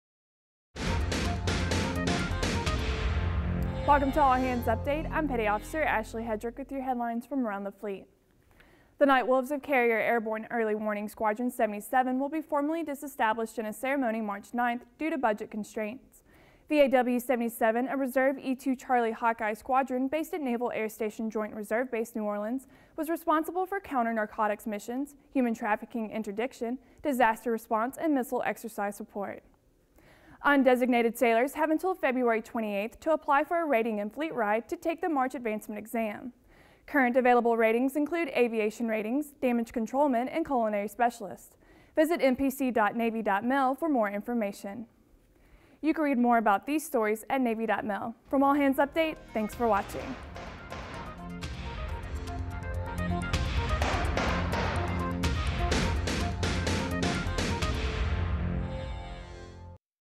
Carrier Airborne Early Warning Squadron 77 to be Disestablished; PACT Sailors
Carrier Airborne Early Warning Squadron 77 to be Disestablished; PACT Sailors Can Still Test for Rating